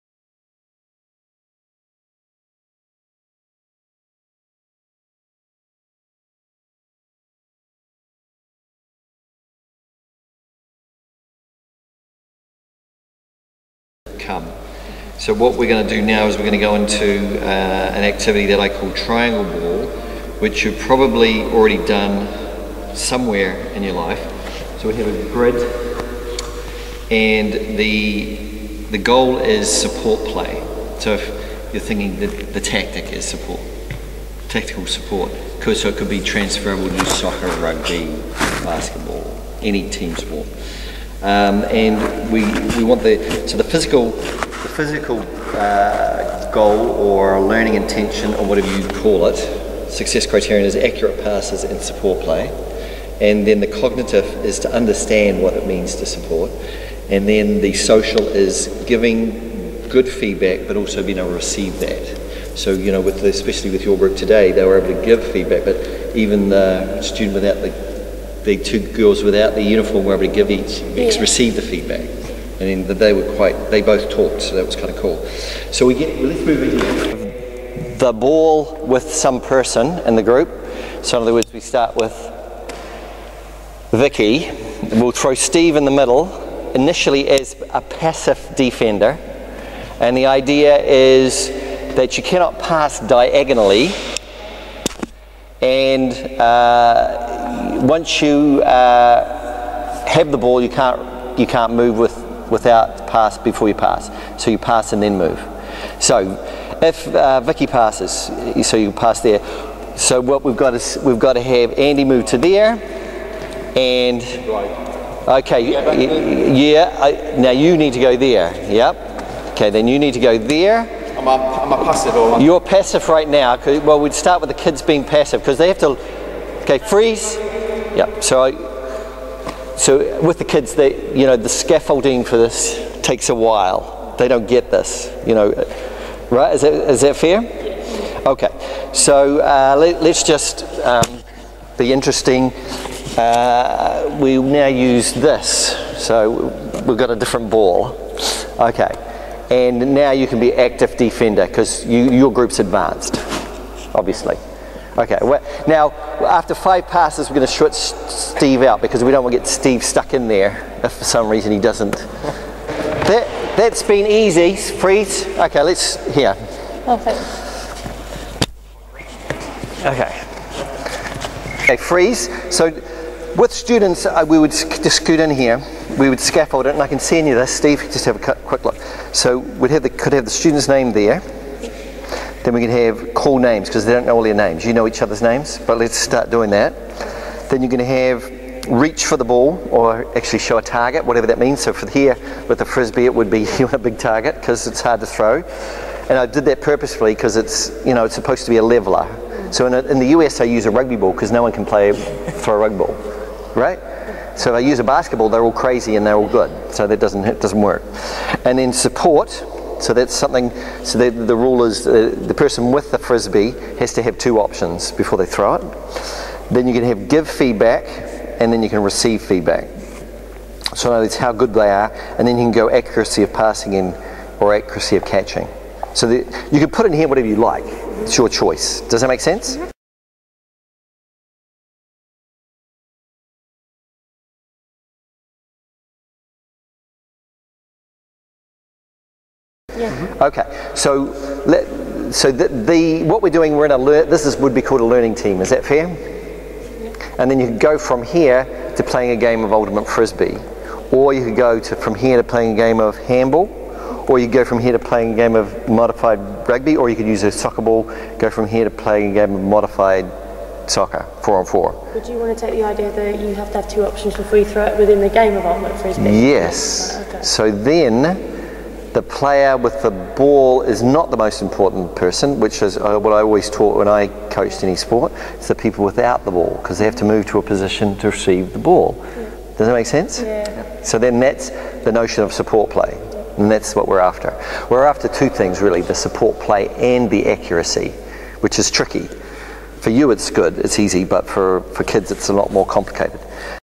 Cooperative Learning Scaffolding: Traingle Ball Task
This activity is an example of a task that can be used within Cooperative Learning.  The other videos within this collection demonstrate how different structures within learning teams can be used to support students learning.

See structures within learning teams part 1 and part 2